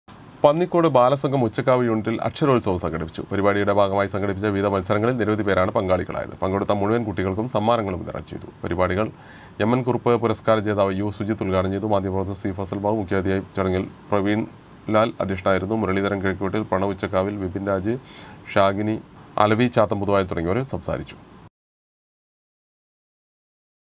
പന്നിക്കോട് ബാലസംഘം ഉച്ചക്കാവ് യൂണിറ്റിൽ അക്ഷരോത്സവം സംഘടിപ്പിച്ചു.
പന്നിക്കോട് ബാലസംഘം ഉച്ചക്കാവ് യൂണിറ്റിൽ  അക്ഷരോത്സവം സംഘടിപ്പിച്ചു. പരിപാടിയുടെ ഭാഗമായി സംഘടിപ്പിച്ച വിവിധ മത്സരങ്ങളിൽ നിരവധി പേരാണ് പങ്കാളികളായത്. പങ്കെടുത്ത മുഴുവൻ കുട്ടികൾക്കും സമ്മാനങ്ങളും വിതരണം ചെയ്തു